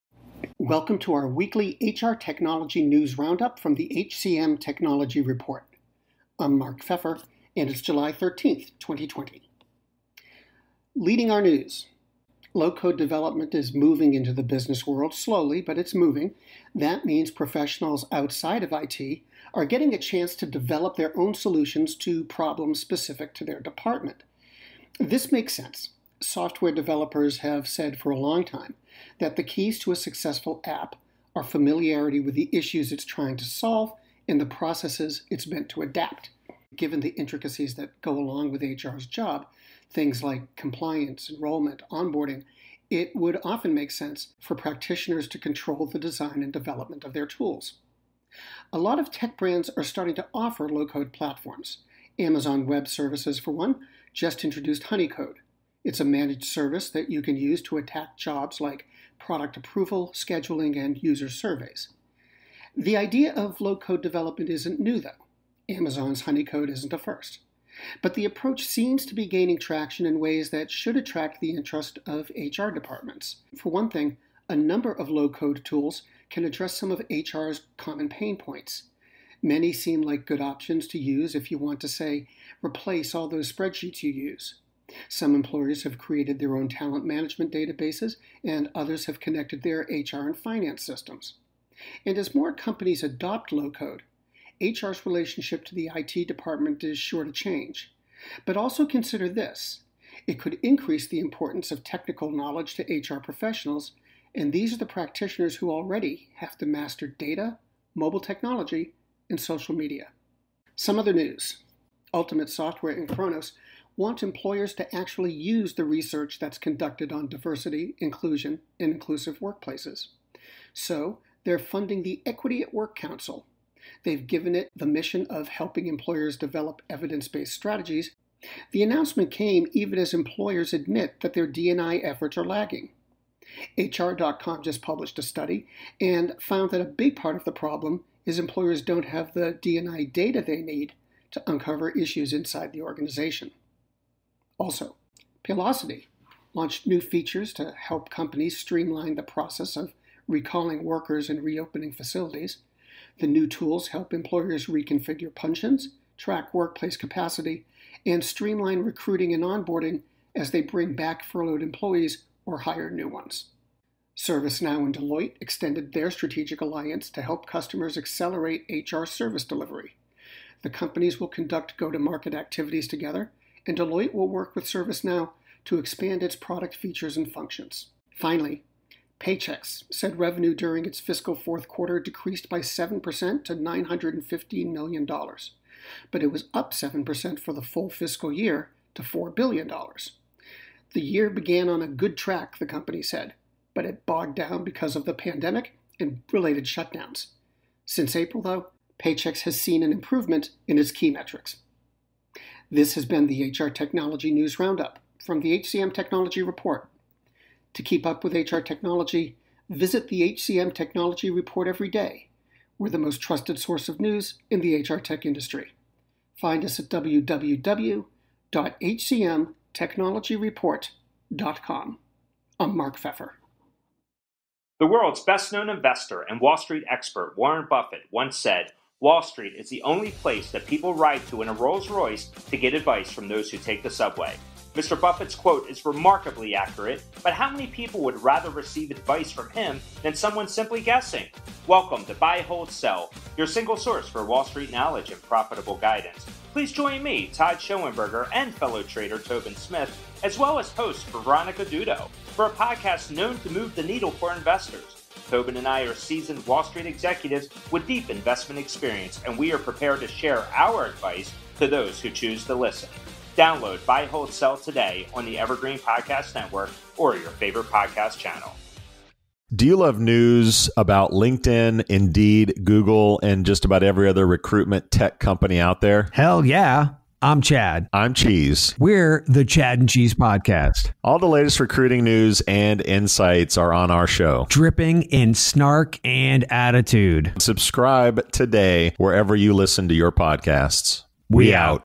July 13, 2020: Low Code Moves into HR; Ultimate, Kronos Fund Equity at Work Council
This week in HR tech news: Low-code development tools begin creeping into HR; Ultimate Software and Kronos want employers to actually use D&I research. And more.